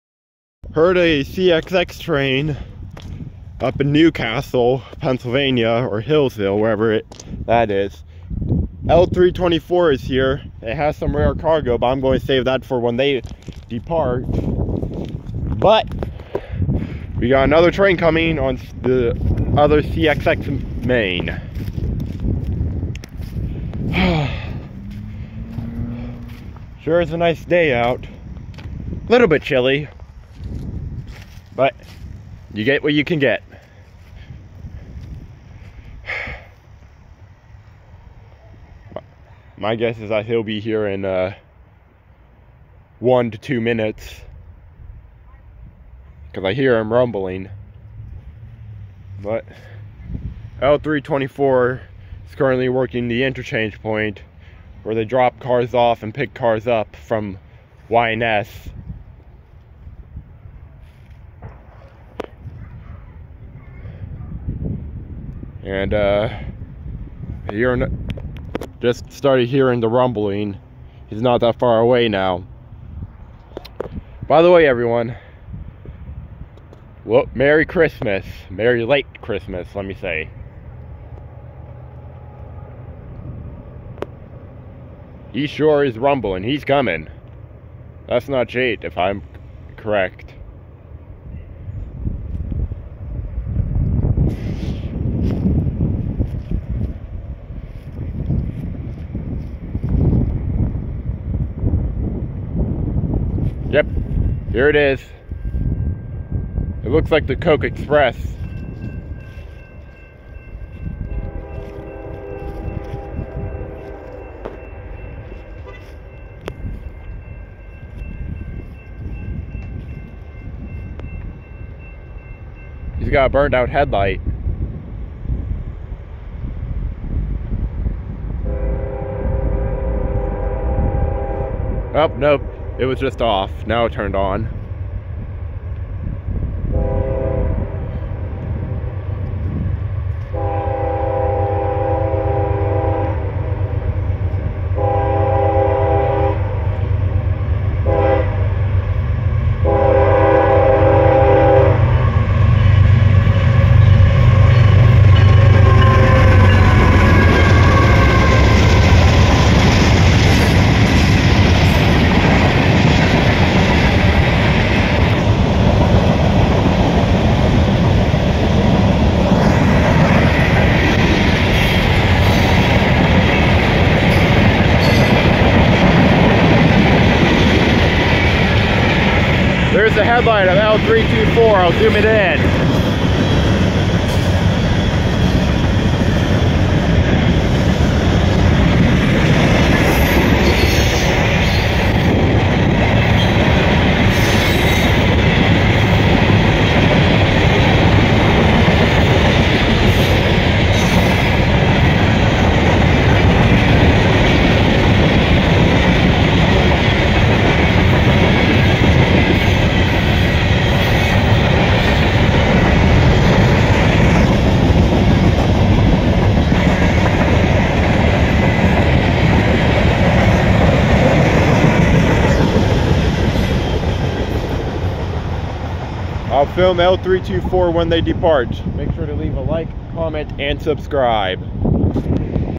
CSX loaded Coke Express passes L324 in Lowellville
second train of the day is a CSX loaded Coke Express passing l324 0324 has something rare so be sure to look out for that video